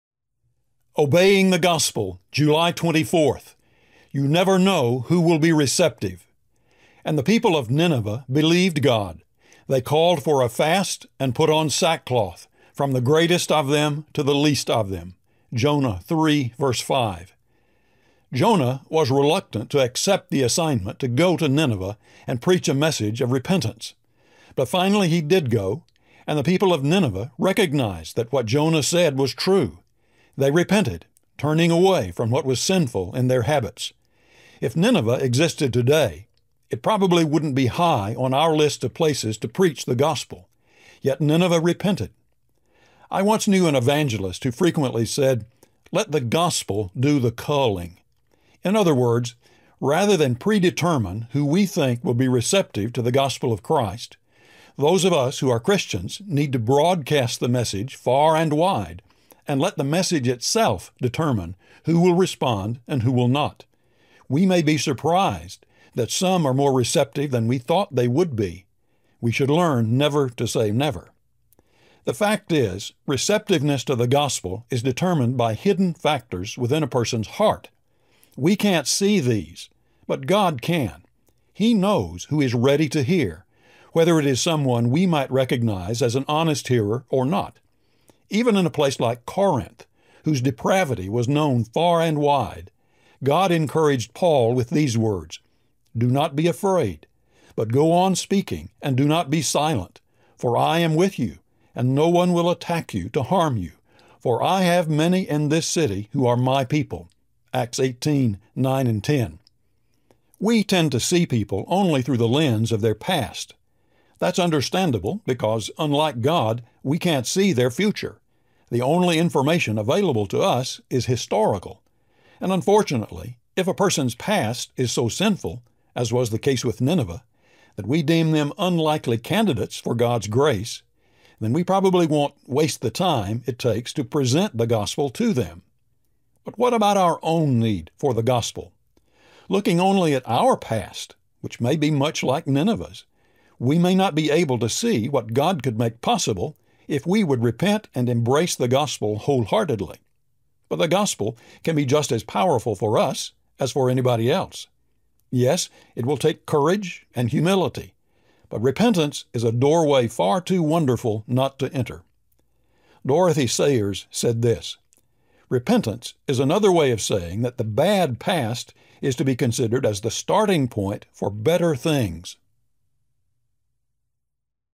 You Never Know Who Will Be Receptive (July 24)
"And the people of Nineveh believed God. They called for a fast and put on sackcloth, from the greatest of them to the least of them" (Jonah 3:5).

JONAH WAS RELUCTANT TO ACCEPT THE ASSIGNMENT TO GO TO NINEVEH AND PREACH A MESSAGE OF REPENTANCE. But finally he did go, and the people of Nineveh recognized that what Jonah said was true. They repented, turning away from what was sinful in their habits. If Nineveh existed today, it probably wouldn’t be high on our list of places to preach the gospel — yet Nineveh repented.

I once knew an evangelist who frequently said, “Let the gospel do the culling.” In other words, rather than predetermine who we think will be receptive to the gospel of Christ, those of us who are Christians need to broadcast the message far and wide — and let the message itself determine who will respond and who will not. We may be surprised that some are more receptive than we thought they would be. We should learn to “never say never.”

The fact is, receptiveness to the gospel is determined by hidden factors within a person’s heart. We can’t see these, but God can. He knows who is ready to hear, whether it is someone we might recognize as an honest hearer or not. Even in a place like Corinth, whose depravity was known far and wide, God encouraged Paul: “Do not be afraid, but go on speaking and do not be silent, for I am with you, and no one will attack you to harm you, for I have many in this city who are my people” (Acts 18:9,10).

We tend to see people only through the lens of their past. That is understandable because, unlike God, we can’t see their future; the only information available to us is historical. And unfortunately, if a person’s past is so sinful (as was the case with Nineveh) that we deem them unlikely candidates for God’s grace, then we probably won’t “waste the time” it takes to present the gospel to them.

But what about our own need for the gospel? Looking only at our past (which may be much like Nineveh’s), we may not be able to see what God could make possible if we would repent and embrace the gospel wholeheartedly. But the gospel can be just as powerful for us as for anybody else. Yes, it will take courage and humility — but repentance is a doorway far too wonderful not to enter.

"Repentance is another way of saying that the bad past is to be considered as the starting point for better things" (Dorothy L. Sayers).